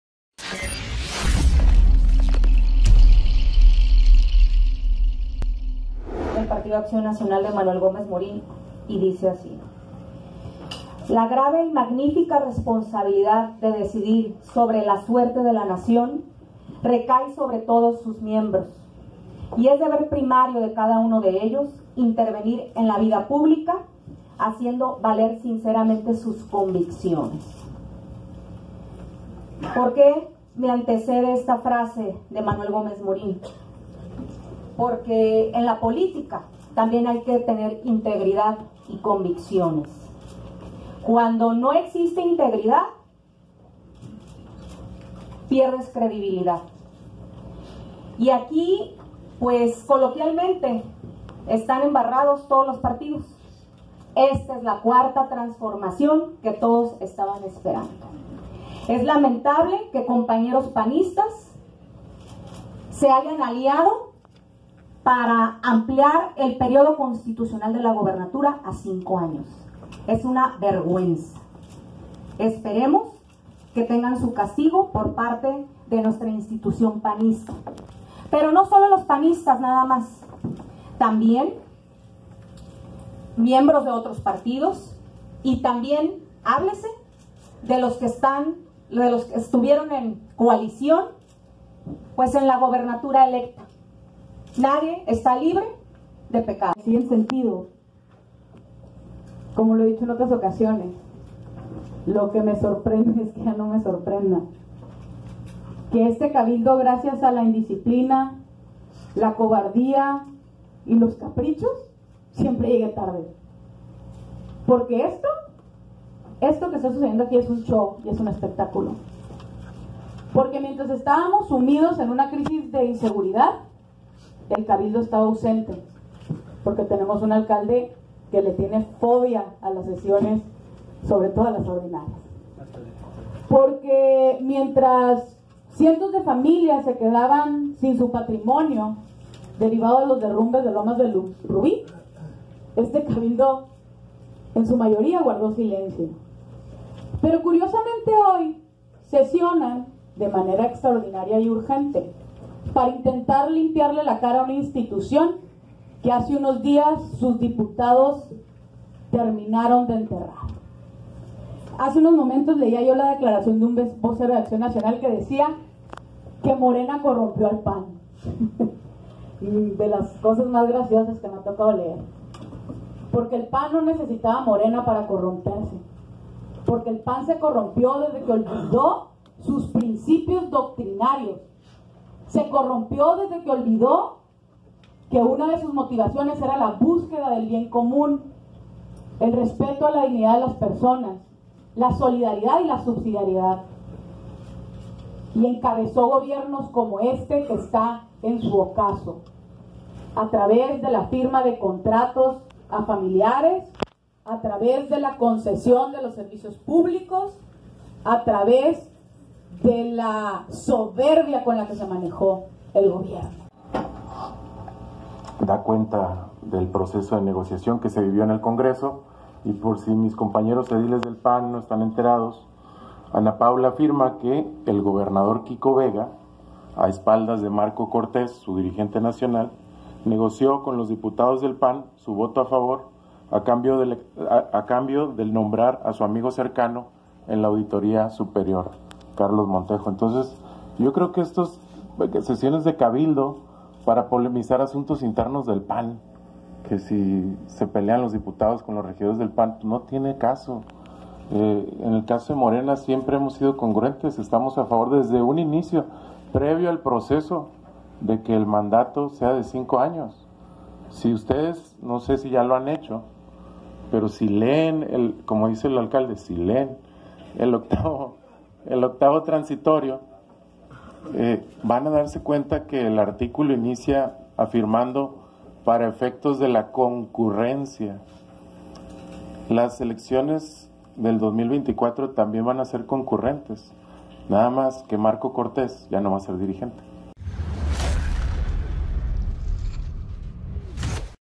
Tijuana vota en contra de ampliación a gubernatura
La mañana de este jueves, siendo el último Ayuntamiento en hacerlo, con 14 votos en contra y dos a favor, el cabildo de Tijuana se pronunció en contra de la ampliación al periodo de la próxima gubernatura, de dos a cinco años, como lo aprobó el pasado lunes el Congreso local.